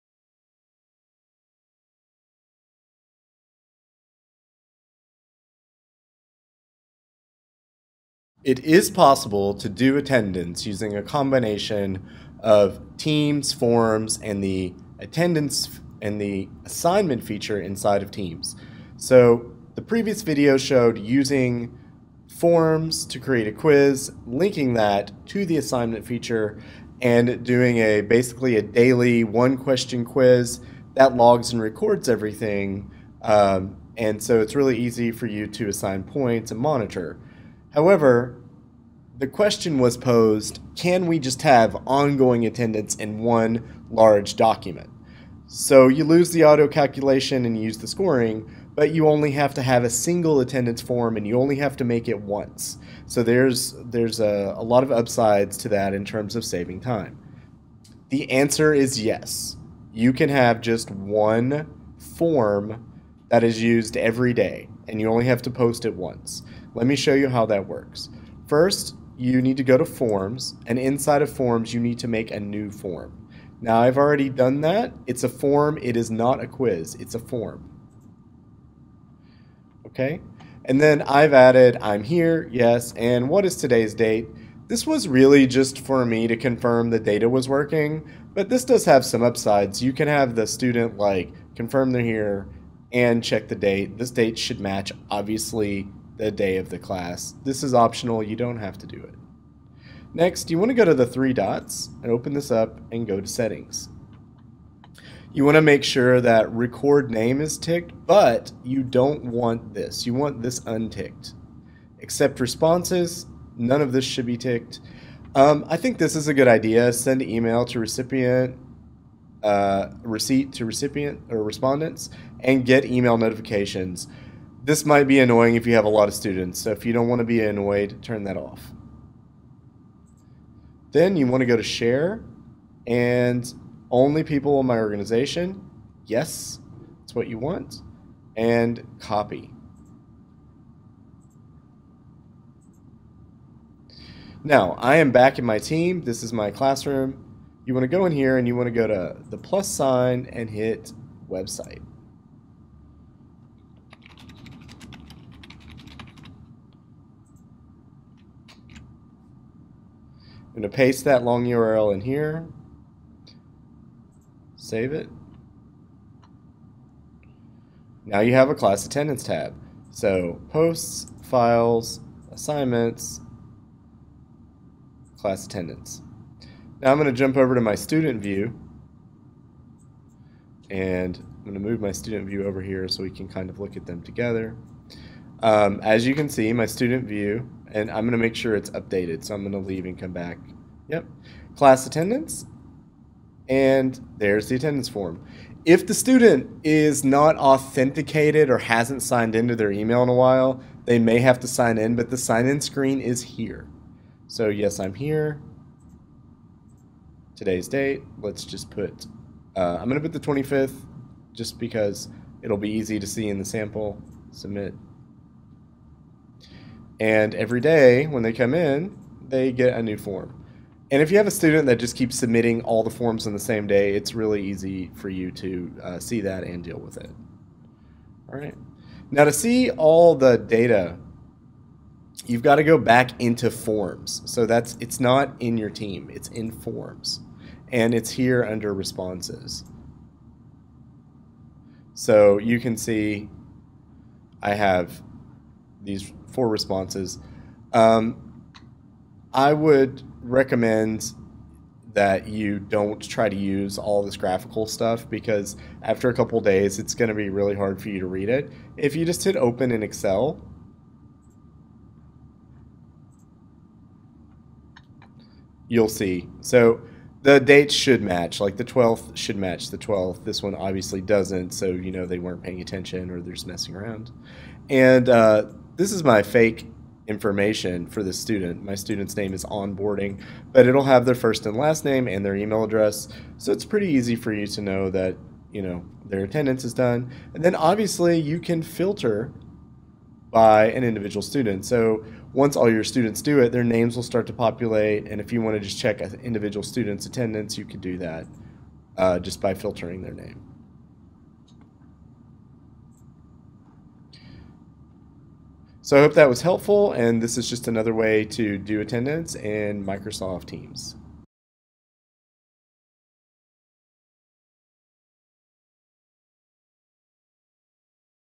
Microsoft Teams Doing Attendance with One Form for the Semester ITBABBLE No Frills Video
Microsoft Teams Doing Attendance with One Form for the Semester ITBABBLE No Frills Video (One Form to Rule Them All)

These solutions are easy to build in the Education versions of G-Suite and O365.